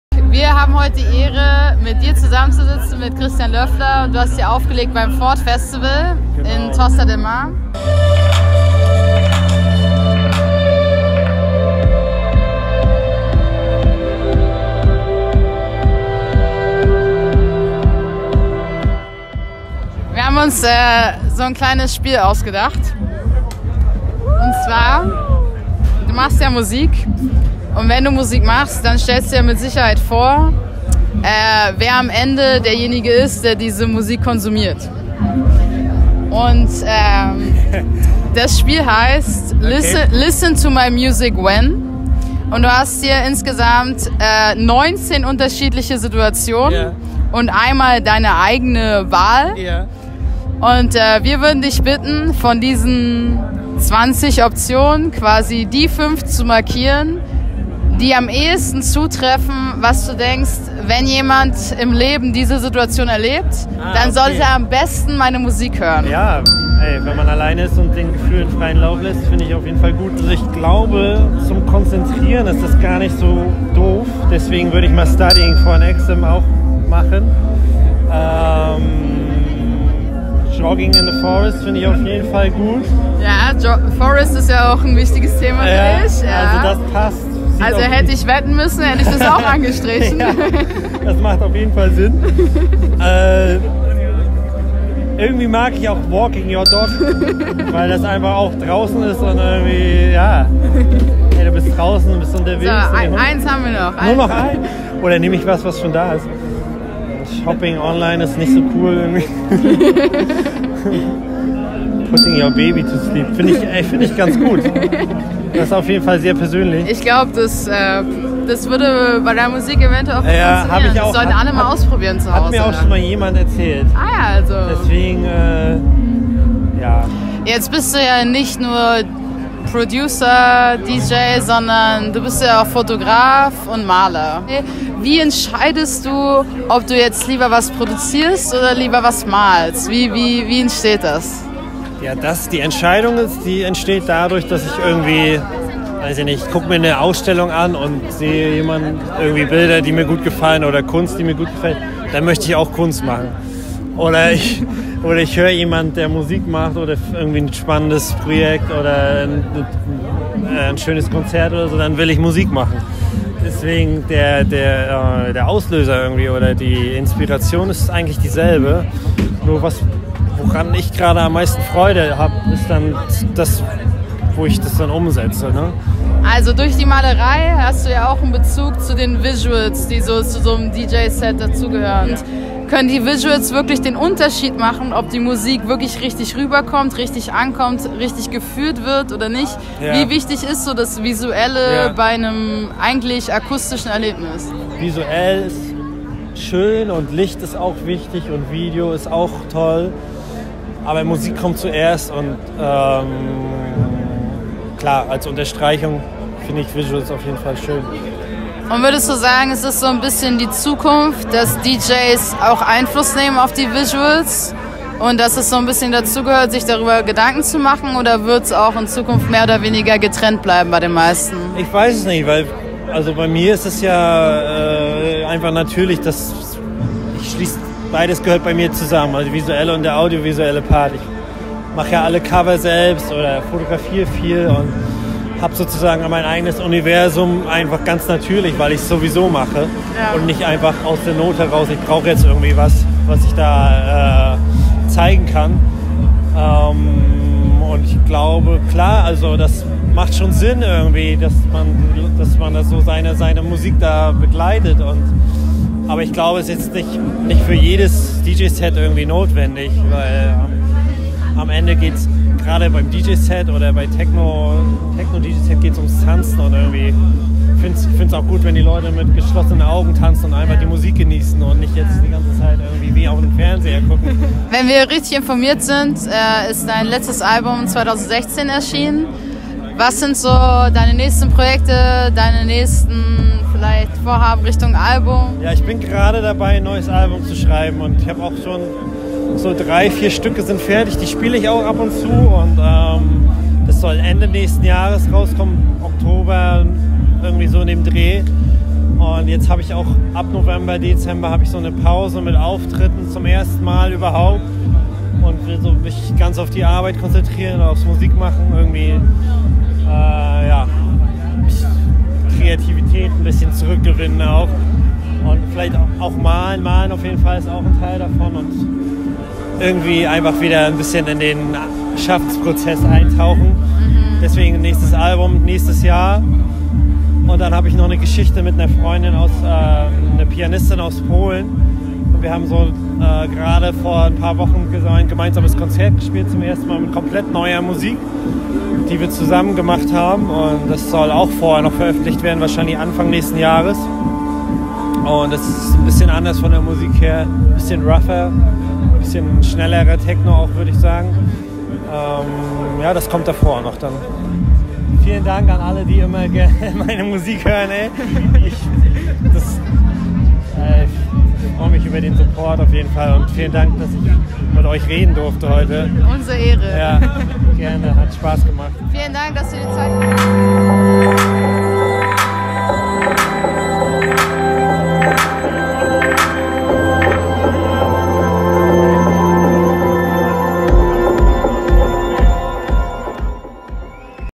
CHRISTIAN LÖFFLER - INTERVIEW | Raverglueck VLog
We were honored to meet Christian Löffler, a German techno and electronica artist who manages to combine melancholy and danceability for a greater audience. We discussed the best situations to listen to his art, his inspiration for music, painting & photography and got a nice outlook on the upcoming projects!